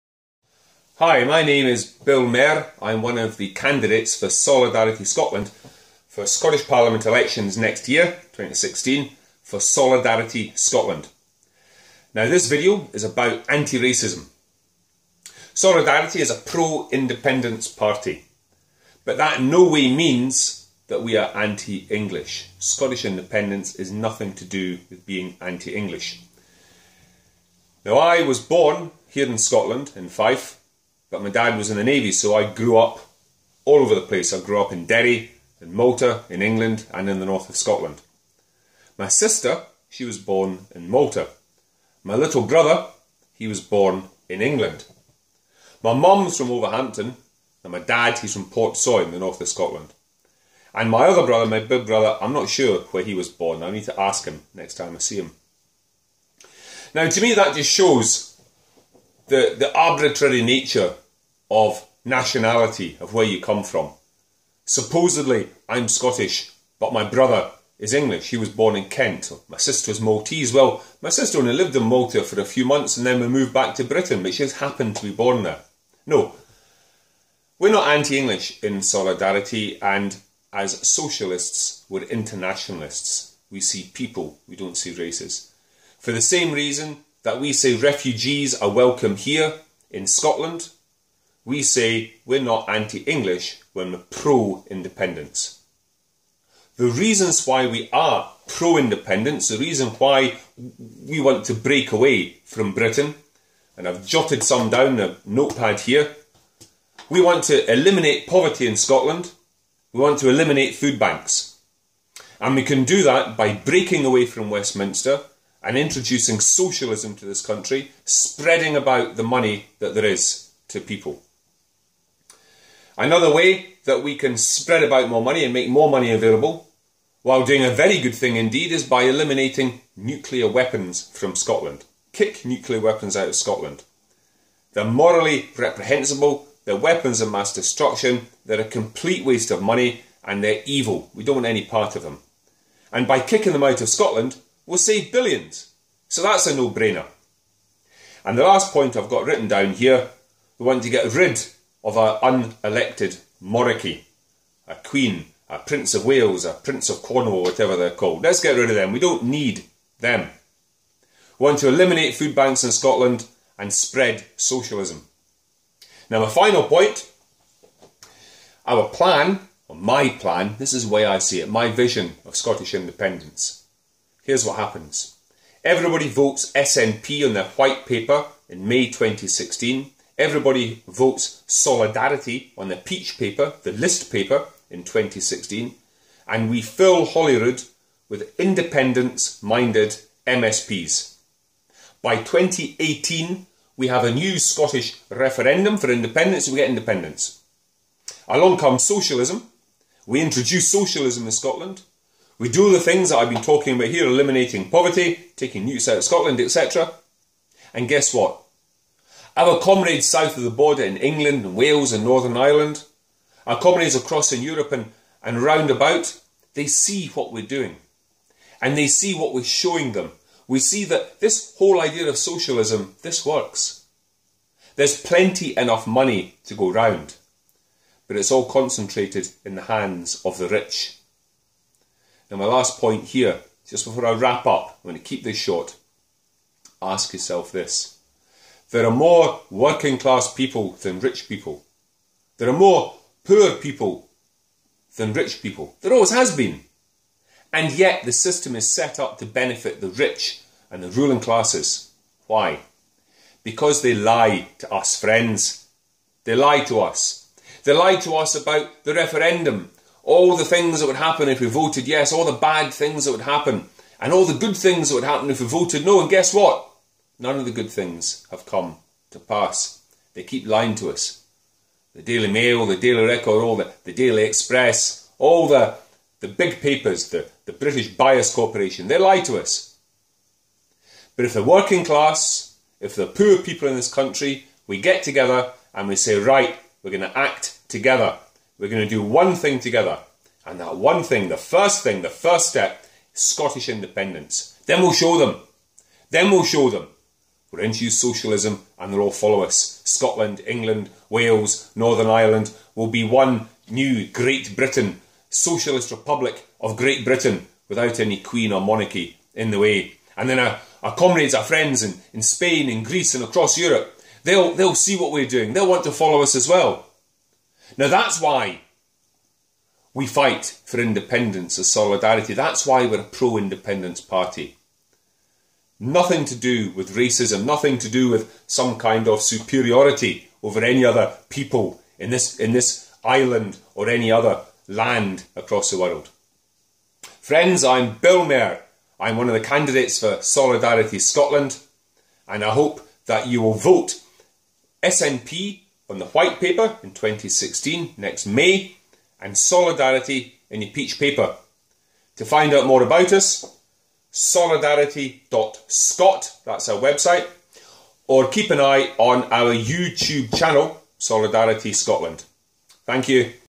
Anti-racist Scottish Independence
Short video by Solidarity candidate Bill Mair, explaining how anti-Englishness has no place in the Scottish Independence movement. (September 2015)